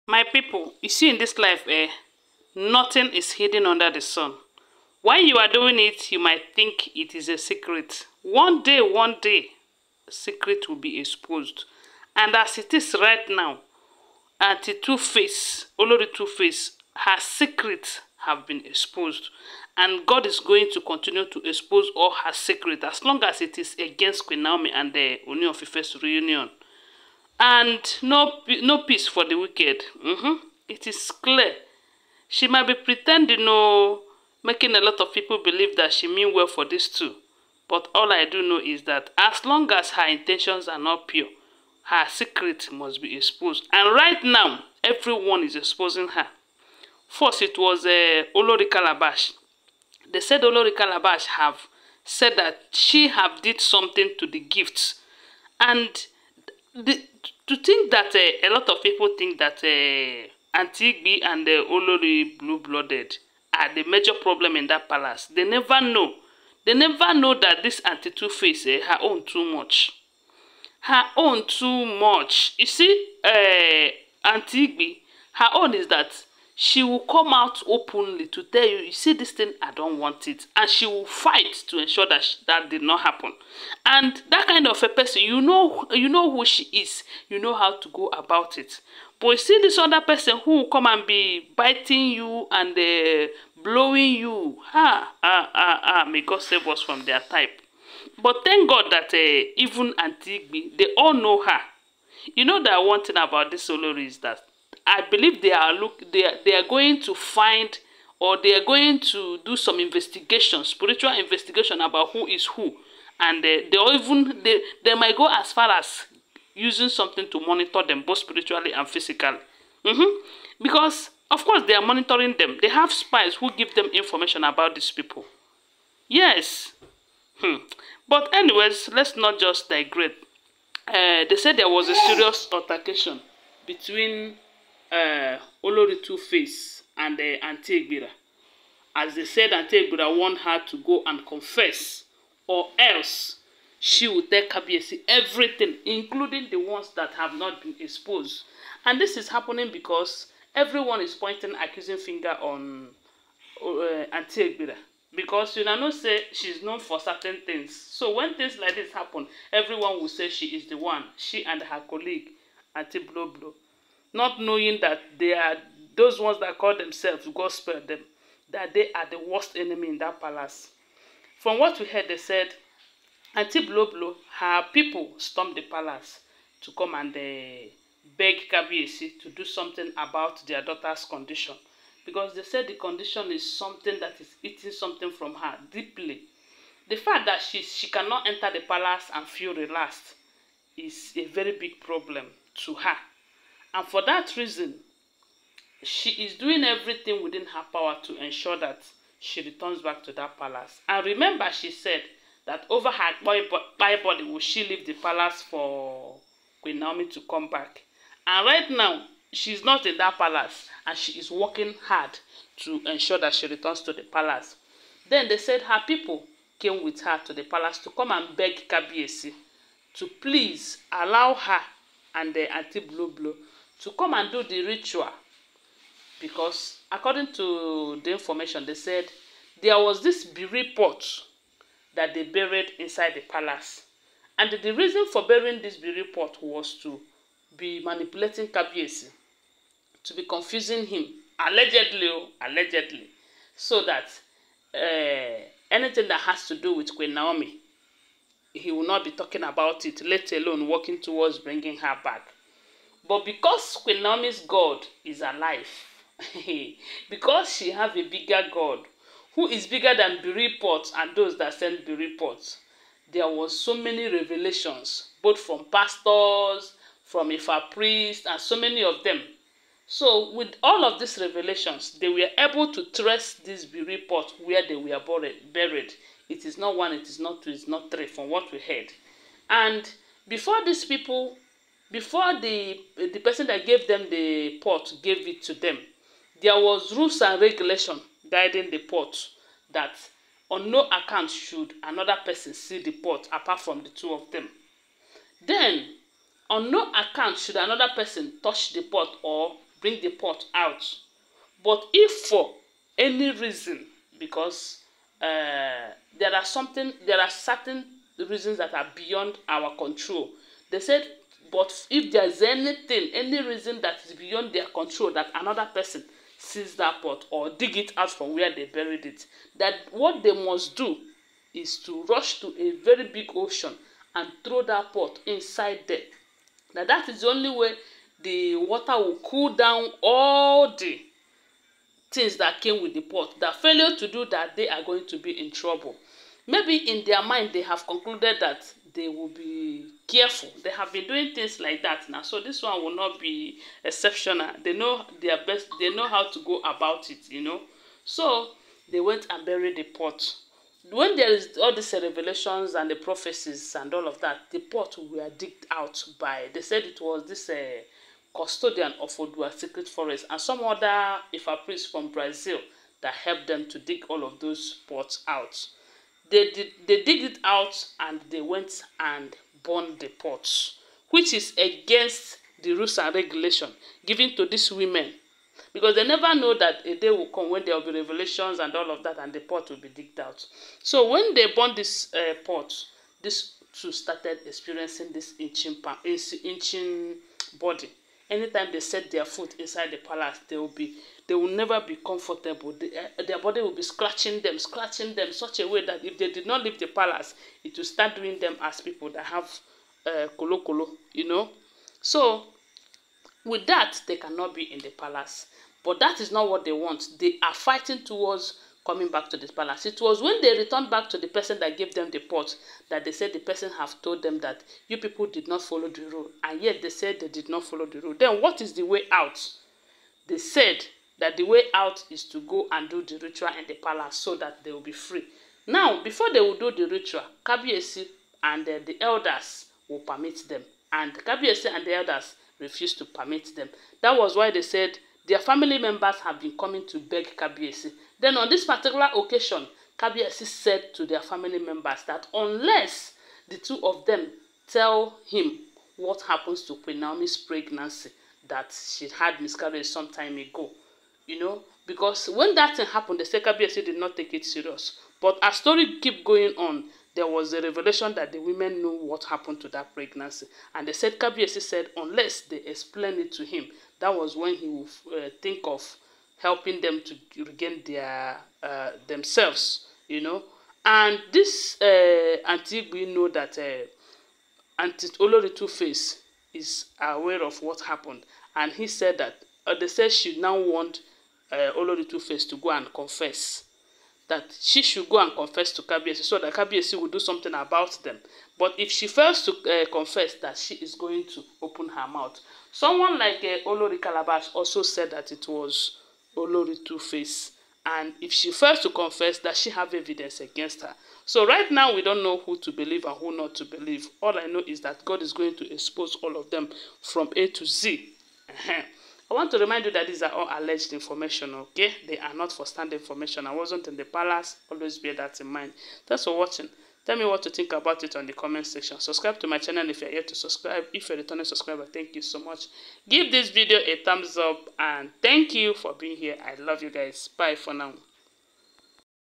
Serious Altércation Between Olori 2face and Anty Ebira As Anty Ebira Wàrn Her To Confess Allegedly..
Serious Altércation Between Olori 2face
 and Anty Ebira As Anty Ebira Wàrn Her To
Confess or She will tell KABIEYESI Allegedly

Ooni of Ife's father commends Dr Elizabeth and Ooni of Ife for Re